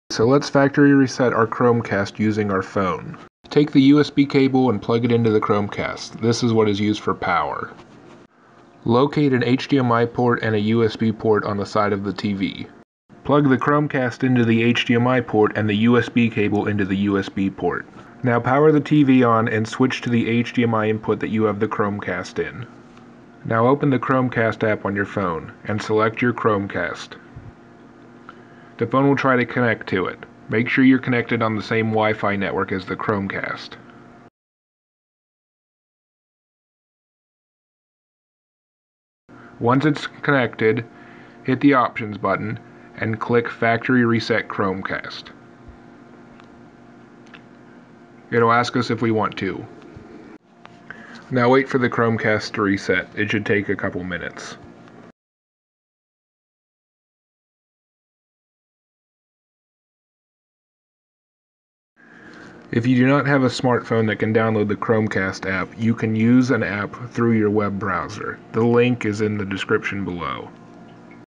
How to reset your chromecast
Difficulty: Easy
If your Chromecast is acting up just reset it. I use an android app in the video but it can be reset using your windows computer as well
You can also hold down the button on the chromecast for 25 seconds.